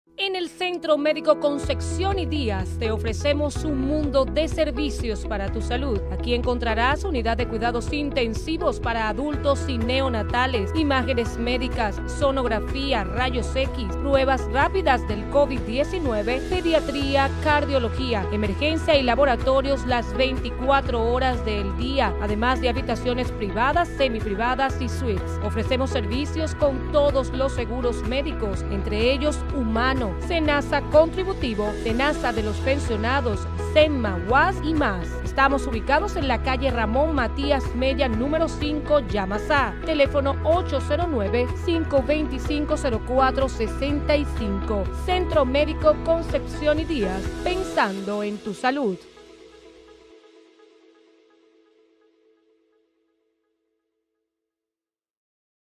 EN EL CENTRO MEDICO CONCEPCIÓN Y DÍAZ TE OFRECEMOS UN MUNDO DE SERVICIO PARA TU SALUD
Publicado el 24 de Febrero del 2021
En el Centro Medico Concepción y Díaz te ofrecemos un Mundo de servicio para tu salud, Encontrara Unidad de cuidado intencivo para adurto, prenatar.
Estaremos respondiendo los comentarios en Intagran, sígueme y déjame tu comentario